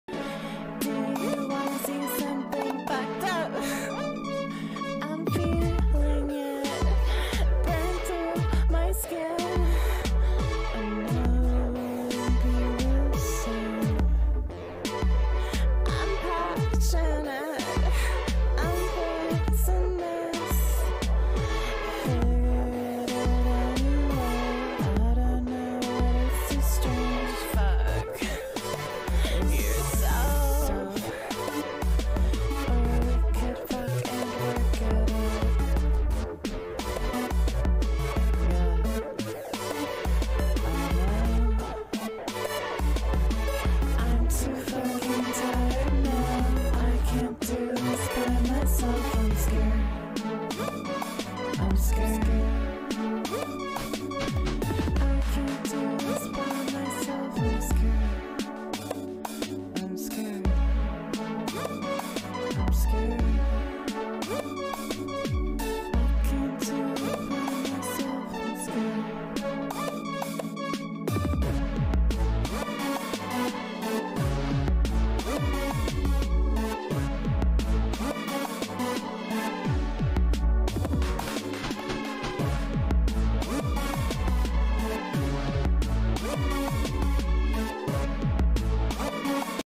COMMISSION | Captain Halley VTuber Demo Video
Showing off a Commission I completed for Captain Halley on Twitter :)
She's a super beautiful Sci-Fi VTuber and I really suggest you check her out!
What better way to test out a model than to lip sync and dance?!

I have FNF stuff there too!